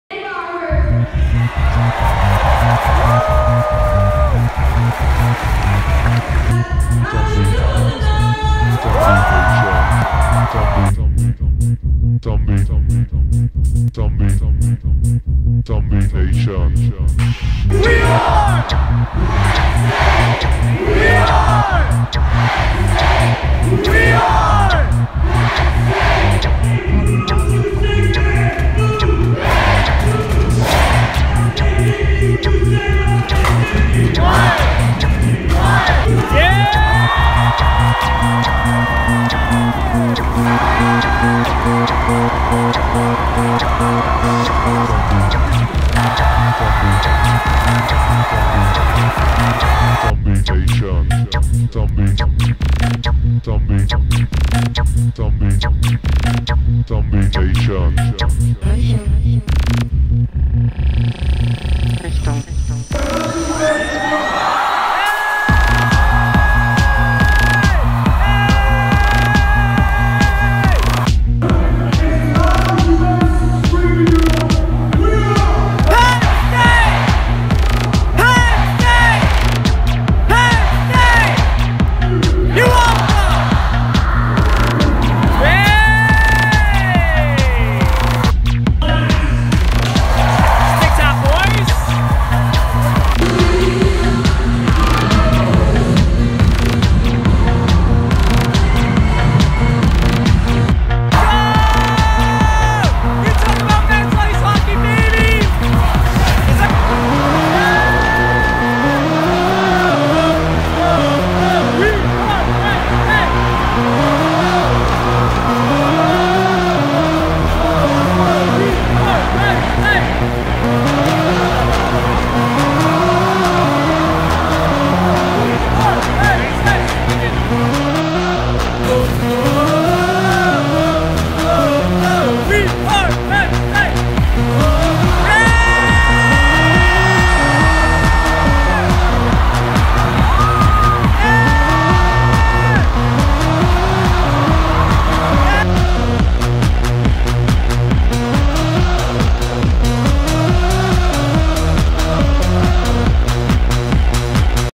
Penn State "Be a Part from the Start" Pep Rally | PSU 22'
Sorry for my awful iPhone camera work. I was too busy getting hyped. The football, hockey, basketball, and volleyball team all came out to fire up the Penn State class of 2022. The WE ARE chants were loud and proud. I am so pumped up and hyped for Penn State. Im so glad I chose this school.